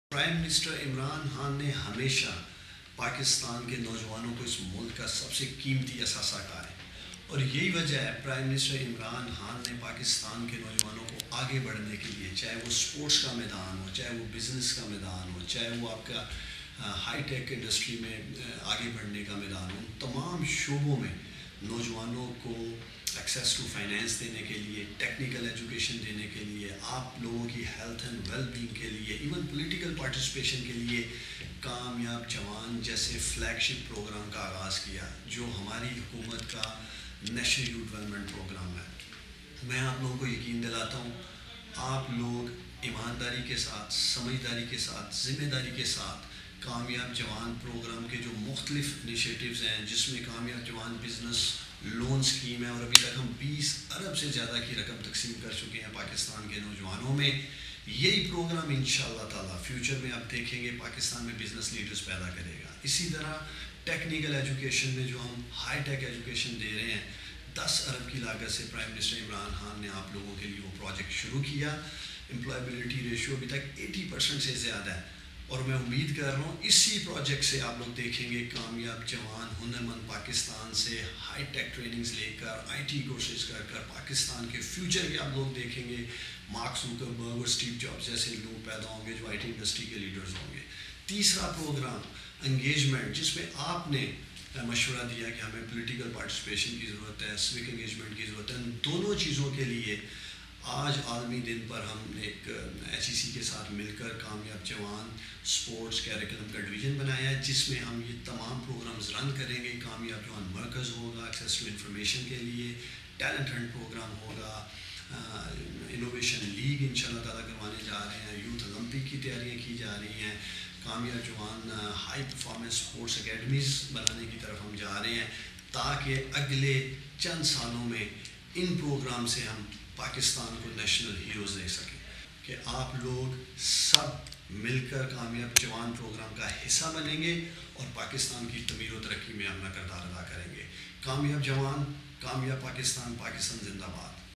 Chairman Kamyab Jawan Programme - Usman Dar's message
Our youth should never lose hope & know that we will keep doing all we can to empower them.